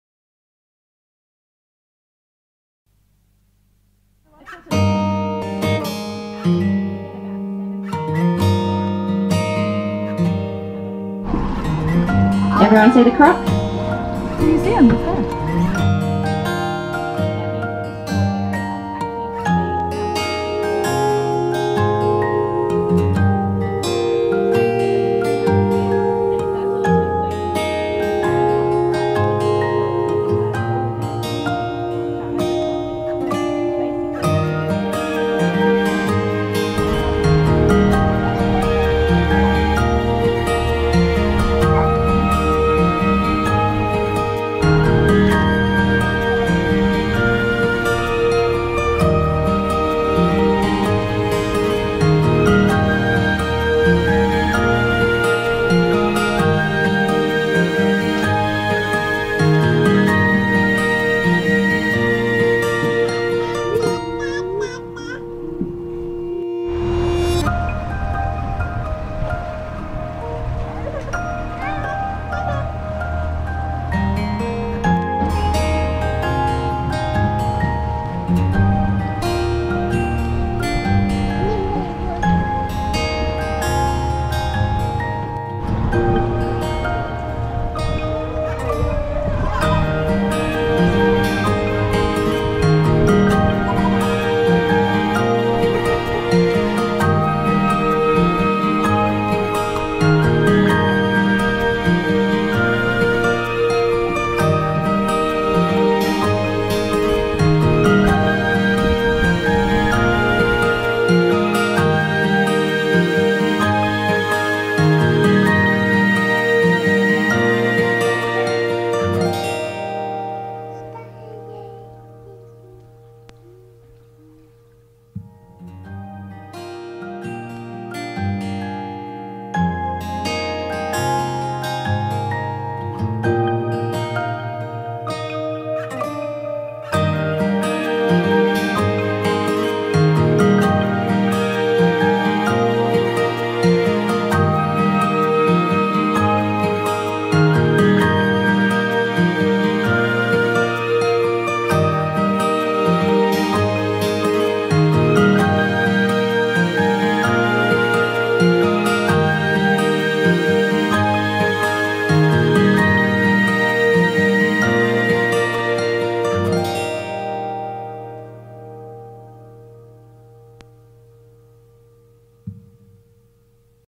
PT54 2022 07 Danngu Gorge WA
Another short video of a beautiful gorge on the Devonian Reef: Dan͟ggu (Geikie) Gorge. This gorge has the Fitzroy River running alongside its walls, to a depth of 30m. The flood level caused by rain water in The Wet (season) is clearly visible on the gorge walls, rising up to 16.5m and completely submerging the visitors info hut and boat tours office pretty much annually!